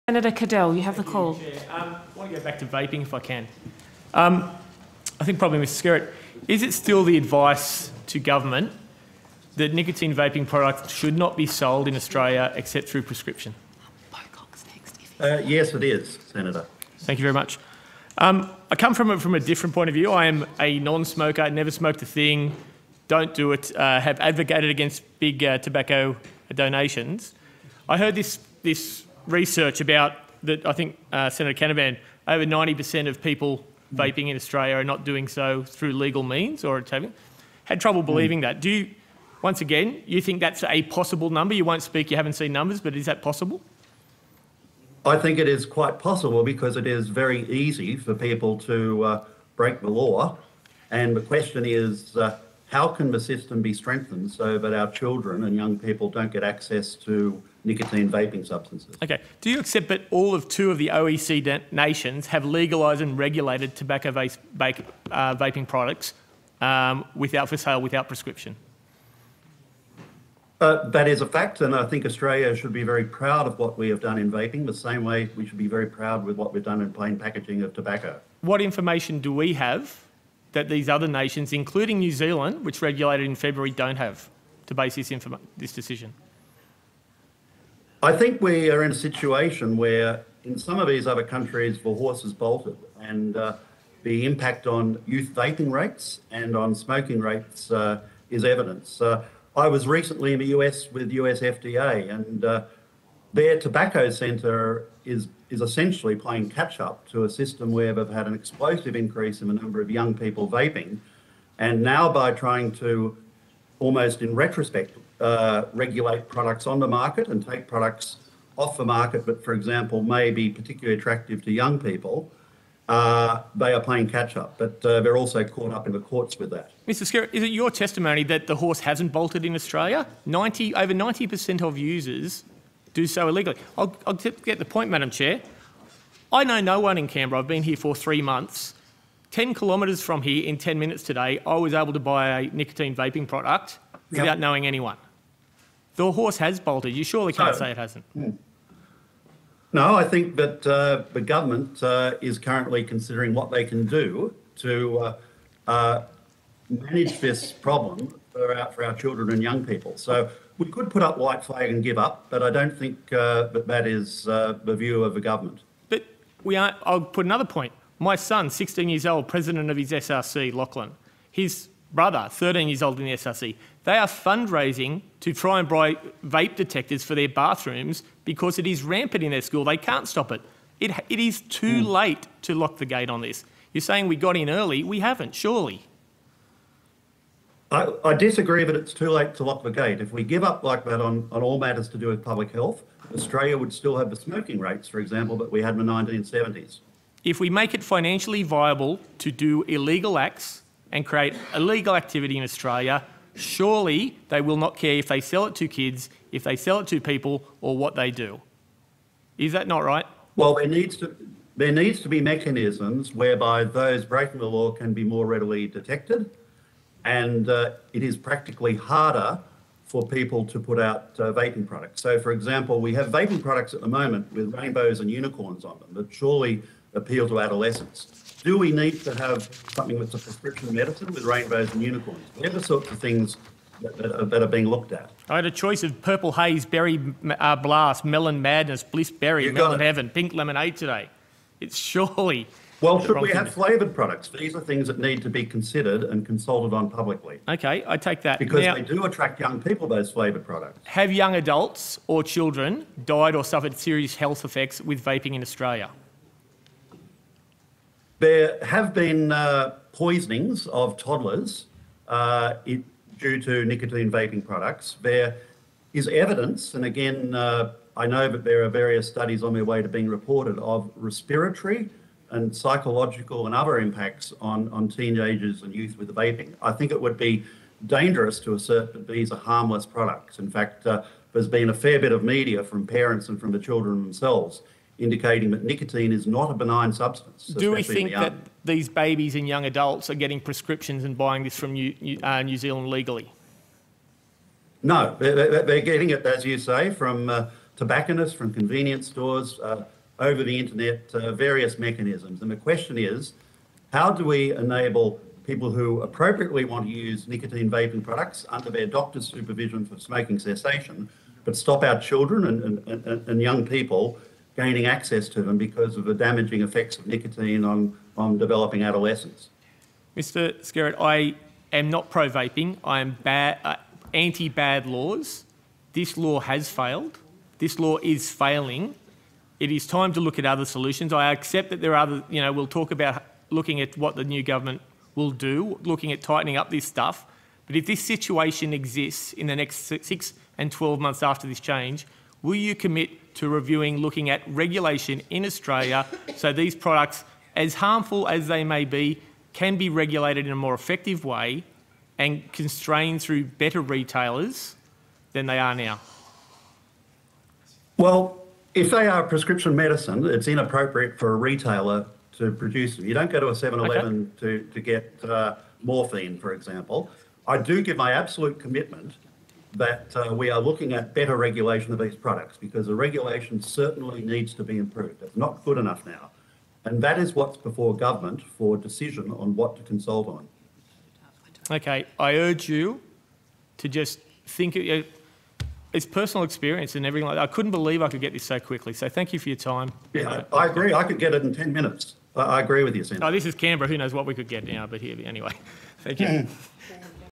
TGA on vaping. Senate Estimate1. 10 November 2022 #1
Senator Ross Cadell
"I am anti-bad laws. This law has failed. It is time to look at other solutions."

(pages 54-57, 60-62)